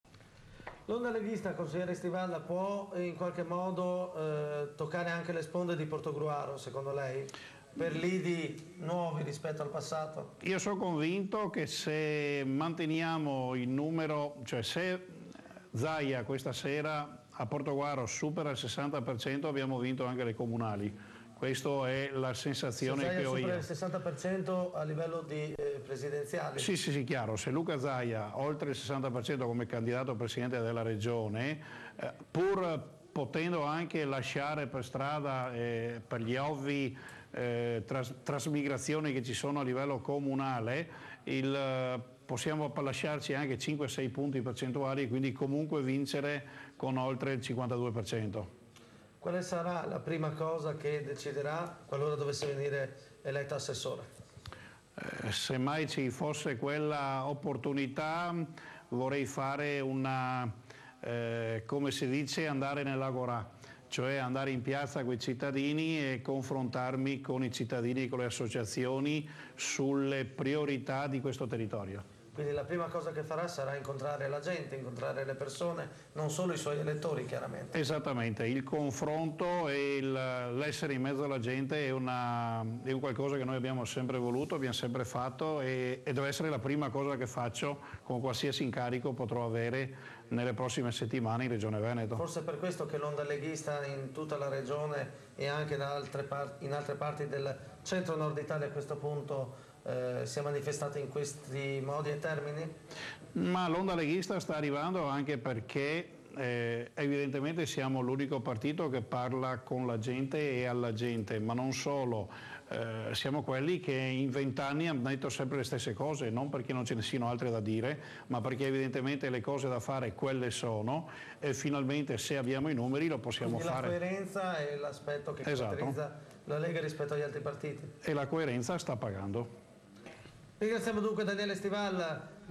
DANIELE STIVAL SBAGLIA PREVISIONI !
Daniele Stival fa  delle previsioni sull'esito del voto finale a Portogruaro, dando  Angelo Tabaro come sicuro sindaco, che ora sappiamo, si sono rivelate completamente sbagliate!